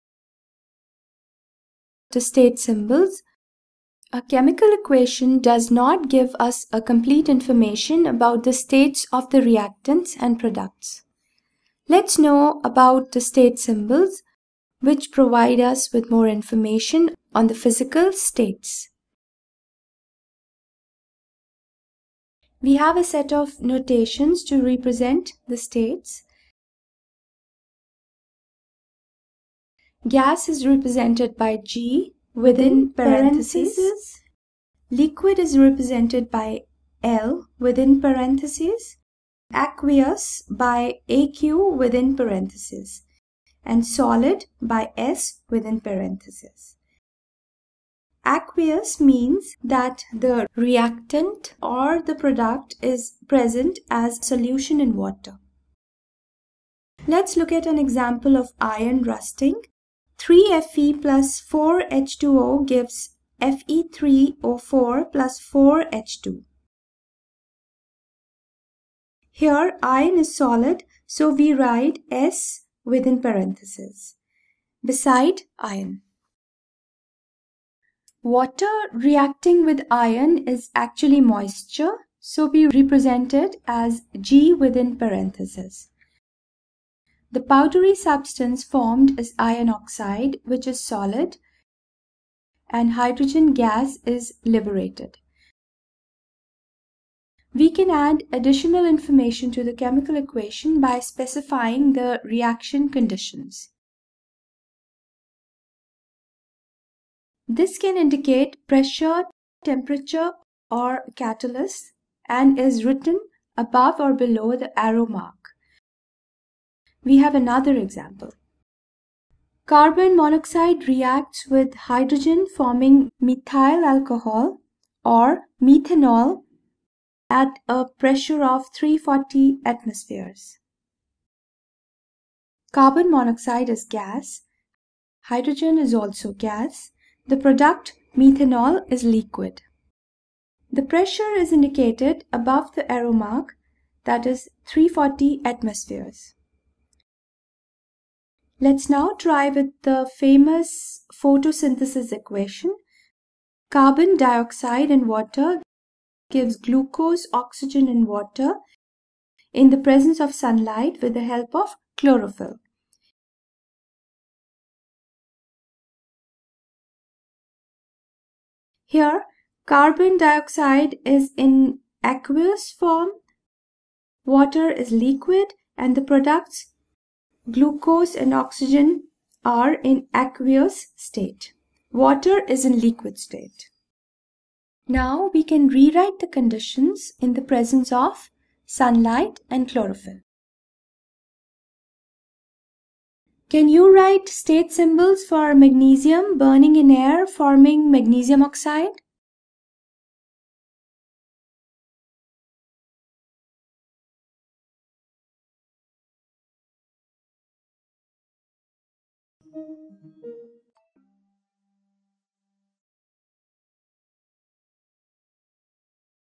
State symbols
Let's include more information to chemical equations using state symbols. We'll learn what state symbols are and their representation with examples. Well also include reaction conditions as additional information to the chemical equations.